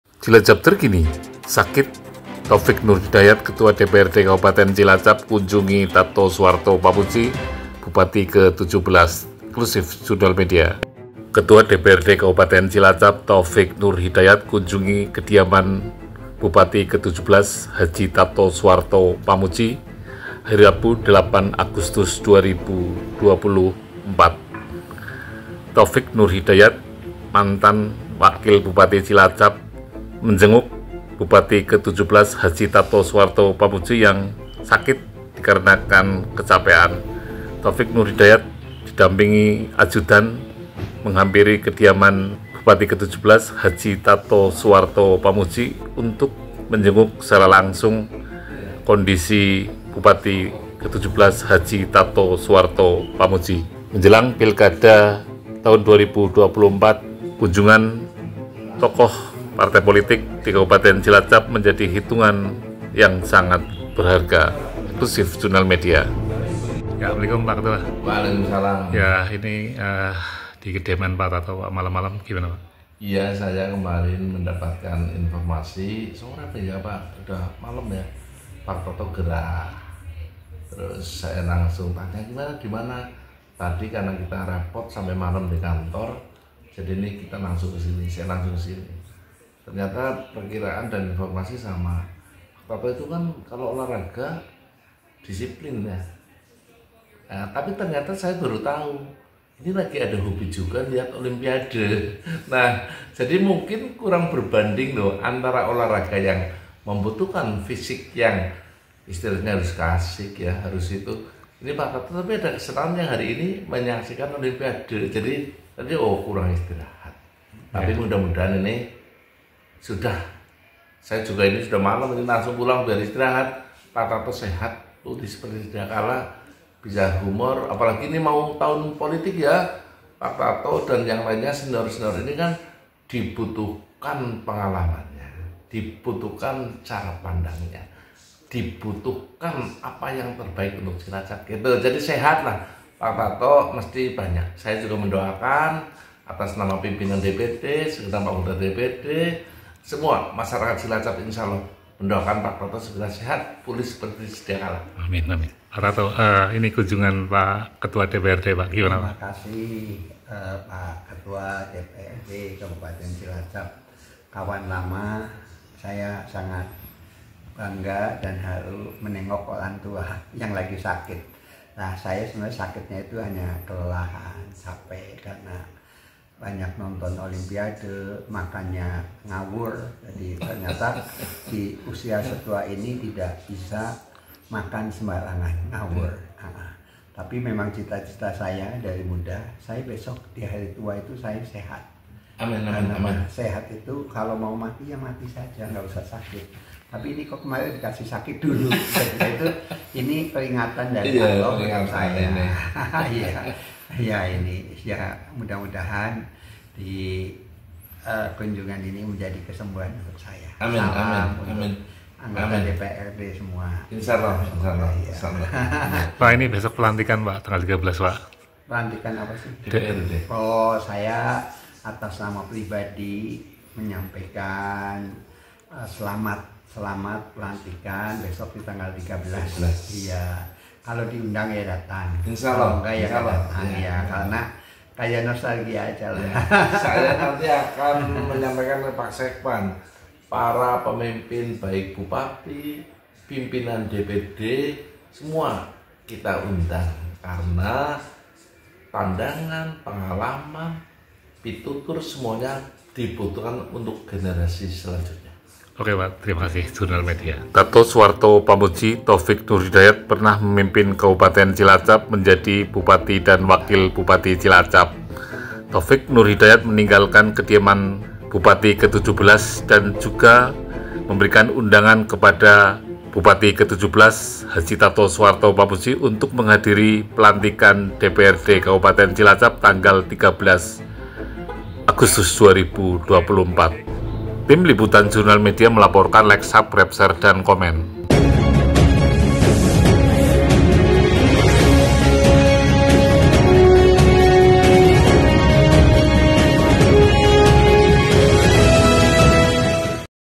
JOUNAL : Sakit, TAUFIK Kunjungi Tato Bupati Cilacap Ke17 | Cilacap Terkini.
H. Tato Suwarto Pamuji Menerima Kunjungan Taufik Nurhidayat Ketua DPRD Cilacap Untuk Menjenguk Paska Rawat Inap di RSUD Cilacap, 8 Agustus 2024.